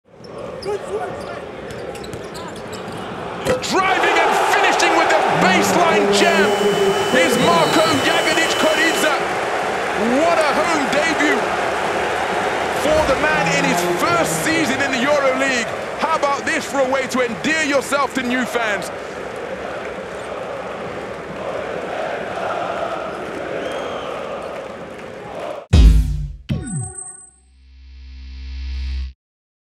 Endesa Dunk of the Night: Marko Jagodic Kuridza, Crvena Zvezda mts Belgrade
Marko Jagodic Kuridza comes off the baseline and throws it down in traffic - and the crowd loves it!